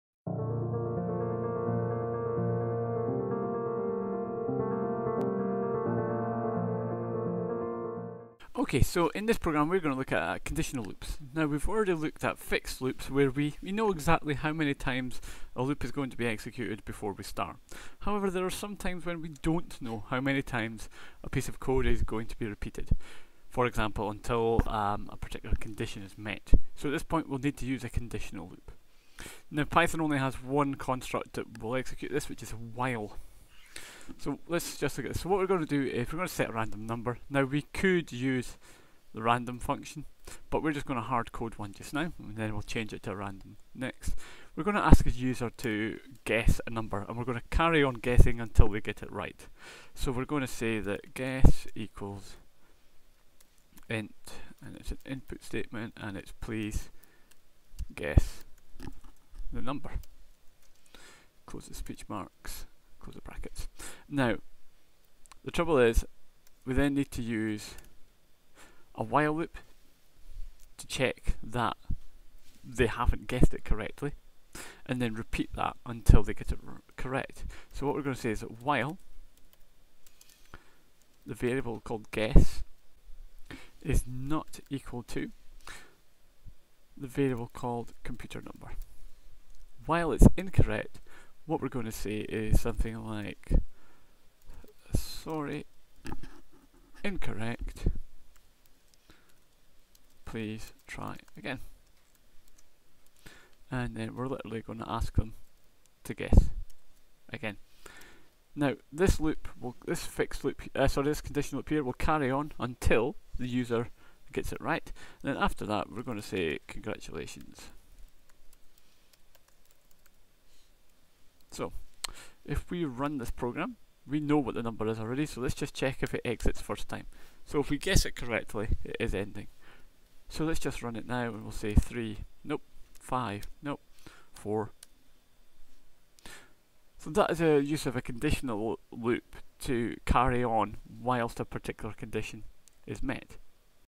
6 Condtional Loops
This covers the basic application of a conditional (while) loop in Python